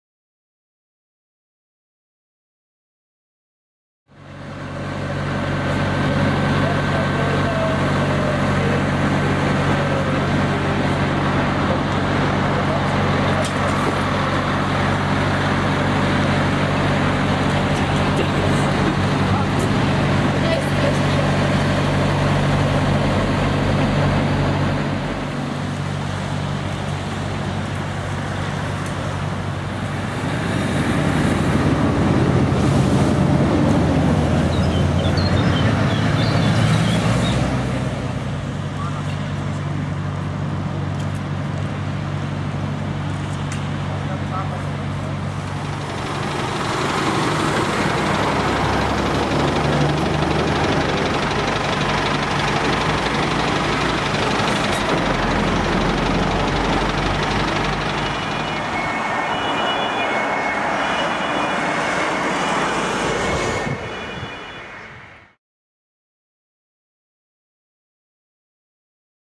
Schwerer LKW Unfall auf der B9 kurz vor Abfahrt Germersheim
Schwerer LKW Unfall auf der B 9 kurz vor der Abfahrt Germersheim. Zwei LKW beteiligt, zwei verletzte Personen. B9 in Fahrtrichtung Germersheim voll gesperrt